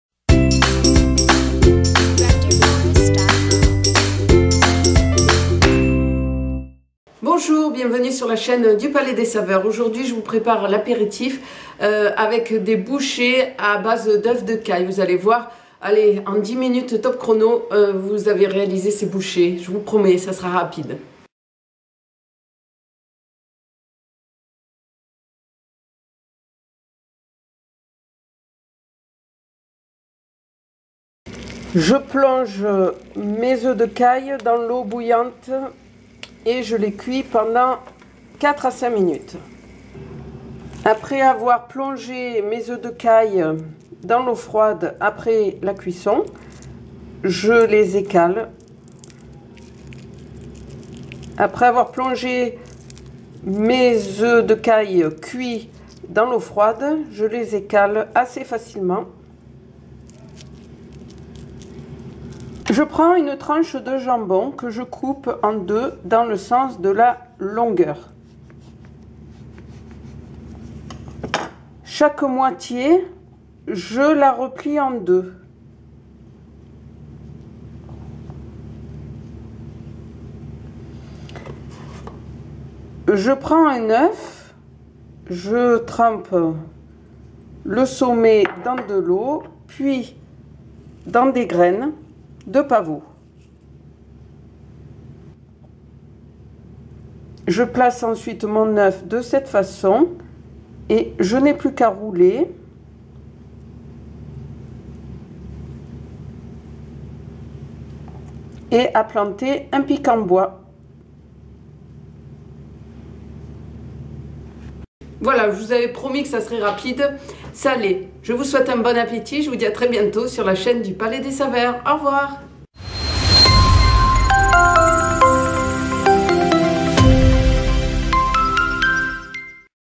Recette des oeufs de caille au jambon et pavot
Voilà une idée qui ravira les plus pressés d'entre vous, avec cette recette de "Un petit oiseau dans la cuisine".